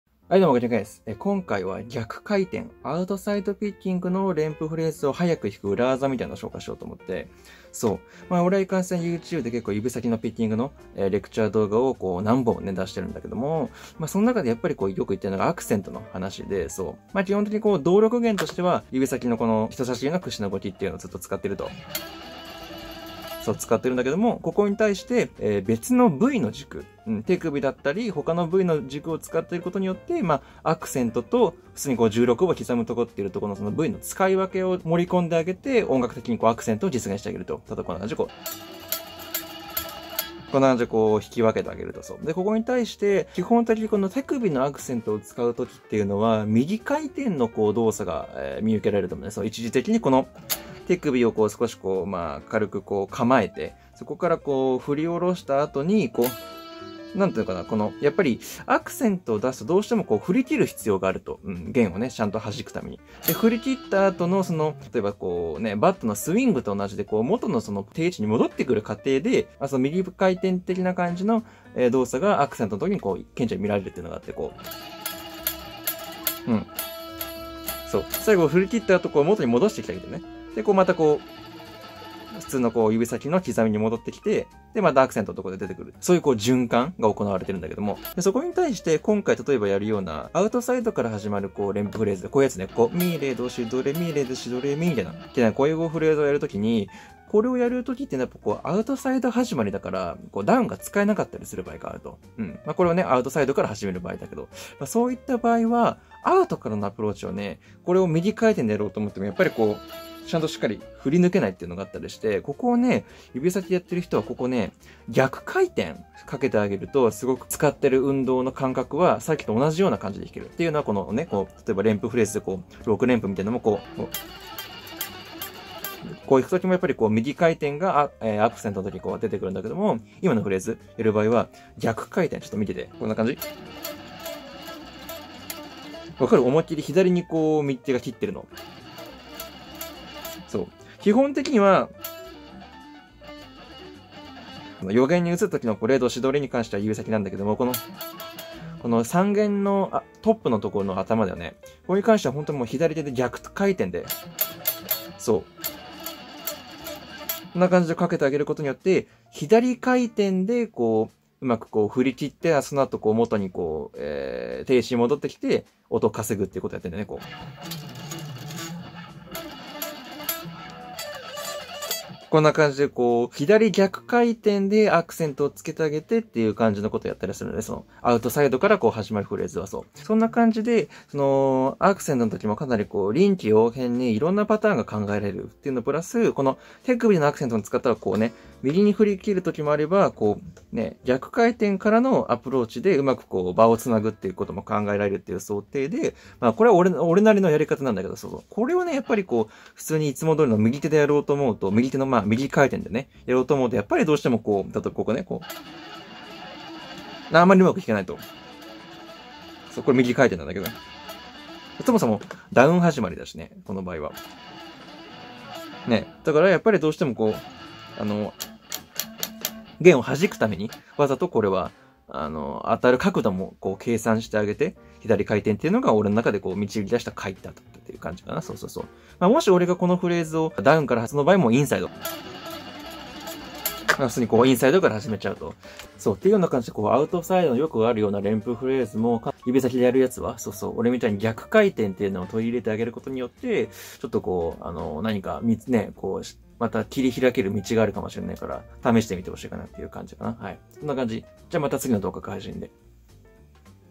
【逆回転】アウトサイド6連符を”左回転”で加速させる裏技。
【guitar】
・Valley Arts Custom Pro USA  ST-Type
・Valley Arts Custom Pro USA  TL-Type
・Valley Arts M SERIES TL-Type
・KG−WISHBONE-V
・Aria Pro II MK-1600

Amazon欲しいものリスト　（生配信用支援物資）

@kaya channel